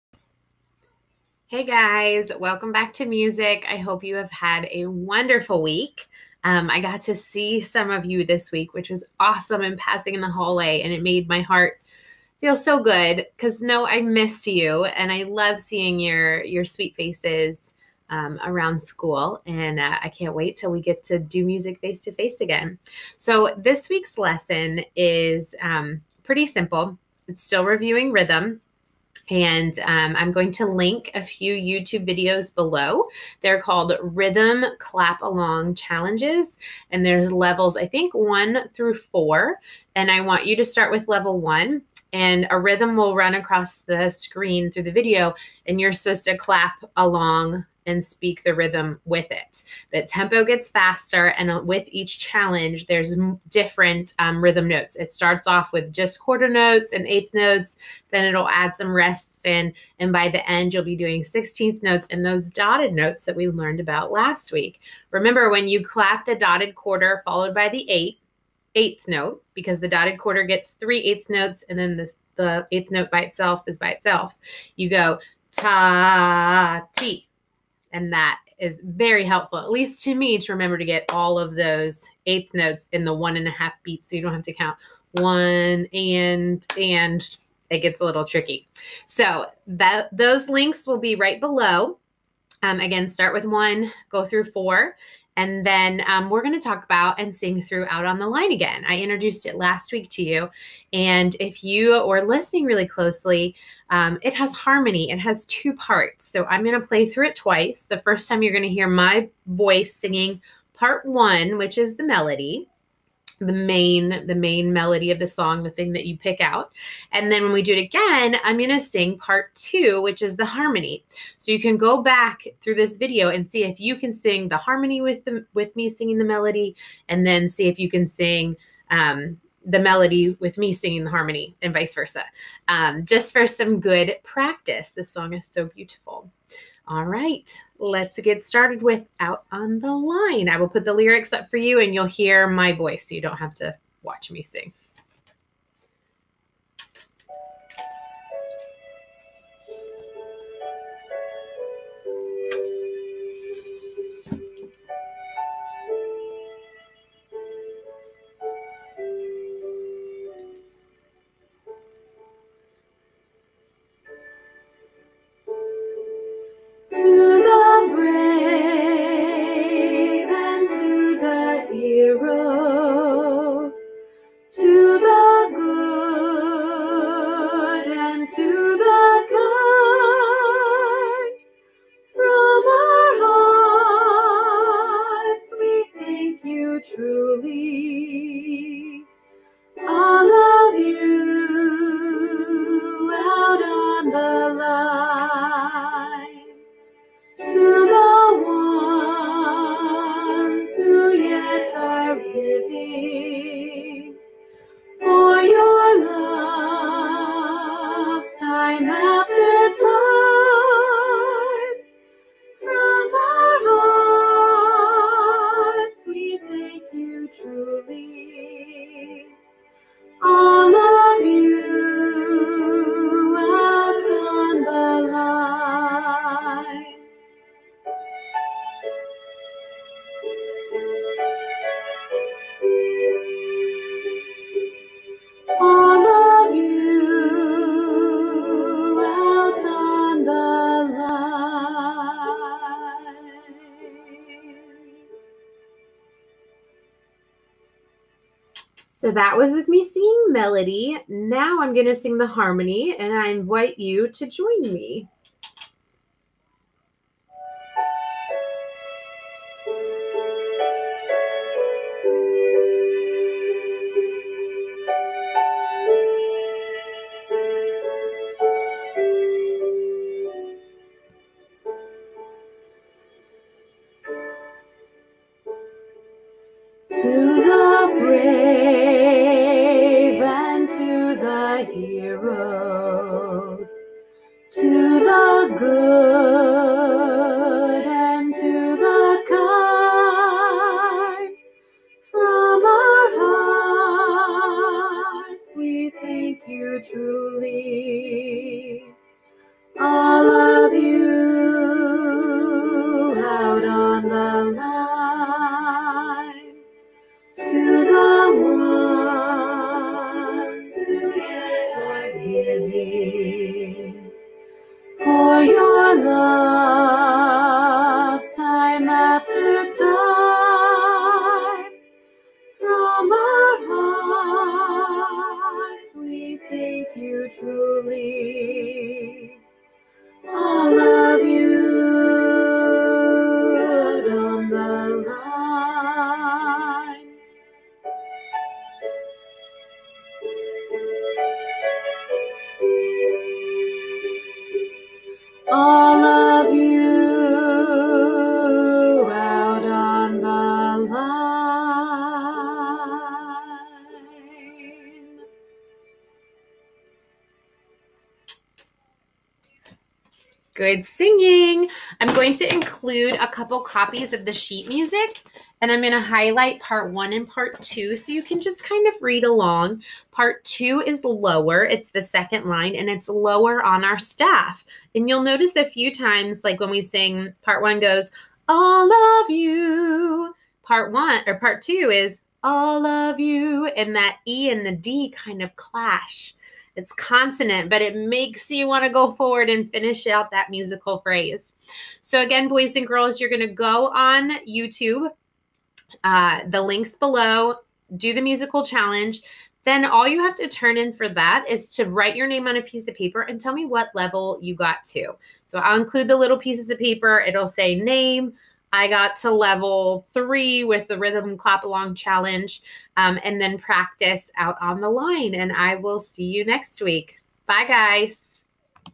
Upper Elementary - Week 4 Assignment Video
“Rhythm Clap Along” challenges.  Start with level 1 and progress through each level you are able to complete.